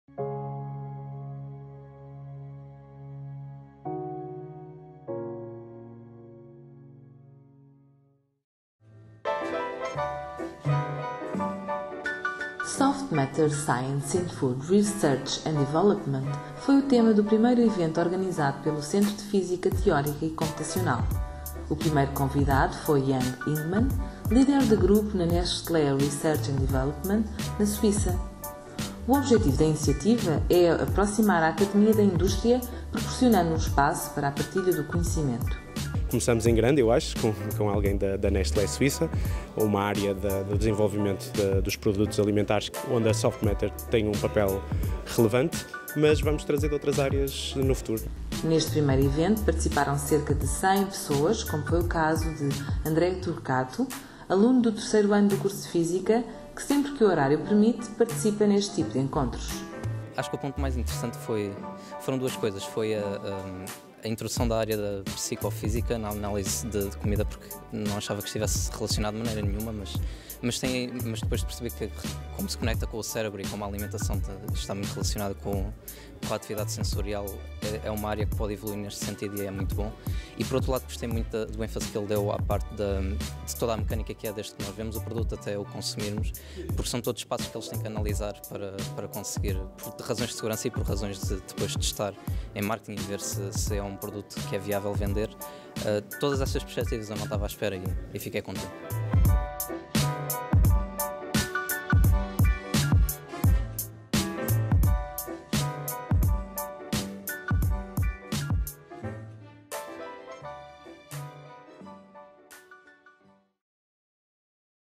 Soft Matter Science in Food Research & Development
A sessão inaugural do ciclo CFTC-Industry Iniciative ocorreu a 26 de novembro de 2018, num dos anfiteatros do edifíco C8 e contou com Jan Engmann, líder de grupo na Nestlé Research Development. A reportagem inclui declarações do professor Nuno Araújo e do aluno André Torcato.